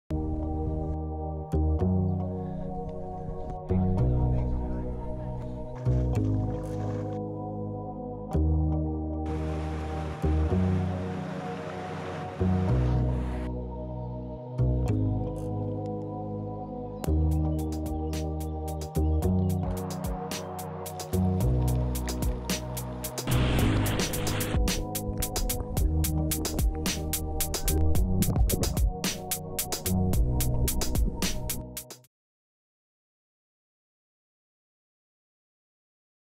My weekend In 30+ seconds
Started with a race up Carrauntoohil , relaxing up the Clydagh Valley and a bbq,  topping it off with my first swim in the Sea this year
Music
csus
Hip hop instrumental